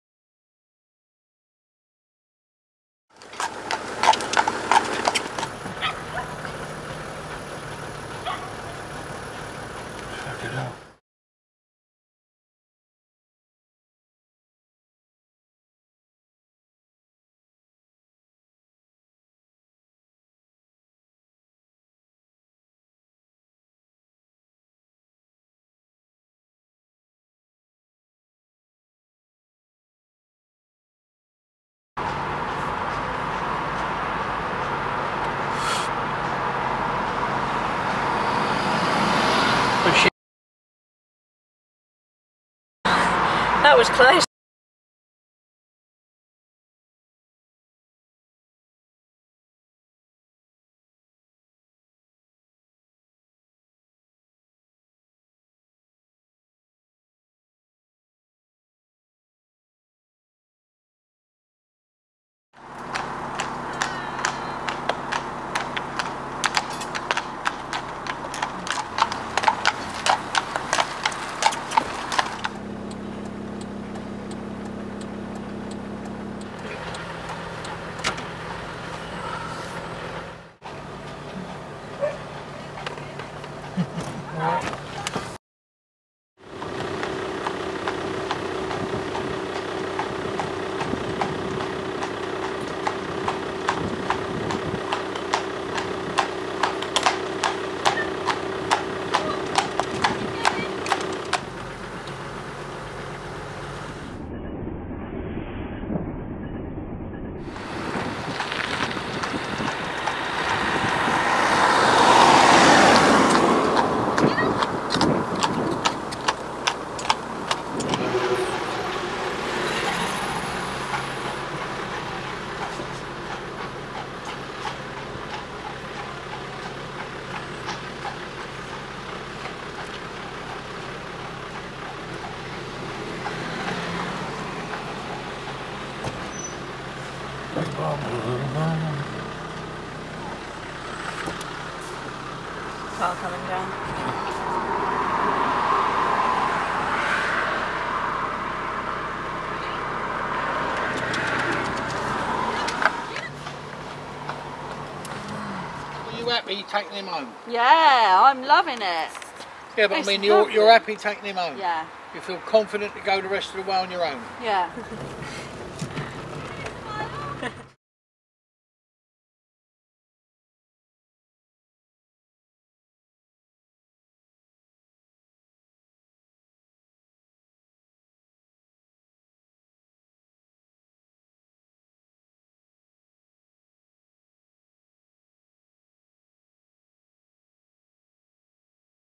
Near miss with car and shire horse! Dangerous driving potential carriage driving accident.
Flynn's owners on the reins. Also showing a near miss when a fast oncoming car and a car overtaking the carriage misjudge their speed/distance. What could have been a carriage driving accident or a road traffic accident was averted due to Flynn's reactions. This sort of unexpected event could happen to anyone, anywhere, at any time on the roads. We believe the roads are for sharing responsibly and therefore as horse trainers it is our job to train horses to cope with the roads, and for lots of things that could happen, not just for when everything is going "perfectly".  Although this unexpected situation was caused entirely by the motorists involved, they could easily have done the same thing overtaking a cyclist, or a slower-moving vehicle such as a tractor, or a parked/broken down car which would also have put not only themselves but their passengers, other drivers and any bystanders at risk if it had resulted in an accident. You cannot ever prevent unexpected things like this from happening, because you cannot ever predict another person's actions (after all, they could be driving under the influence of alcohol or drugs, driving without a license, or in a dangerous car, yet still using the roads). Therefore as responsible horse trainers we are doing all we can to ensure that when we use the roads, our horses will not be the cause of any accidents or incidents due to their behaviour. Whether you are a car driver, cyclist, pedestrian, rider or carriage driver, the roads can be hazardous, but here the horse is well-trained enough to cope with this situation without panicking and turning it into a major disaster. Cars are the more recent addition to our roads, being invented long after the horse-drawn carriage, but rather than blame motorists for their actions and resenting sharing the road with them, we prefer to focus on training our horses so we know we can share the roads happily with anyone who uses them, from cars, ambulances and tractors to pedestrians with umbrellas, barking dogs and cyclists.

We show his owners taking Flynn out on the roads for a drive on their way back to their yard.

Horse Drawn Promotions - Breaking horses to harness.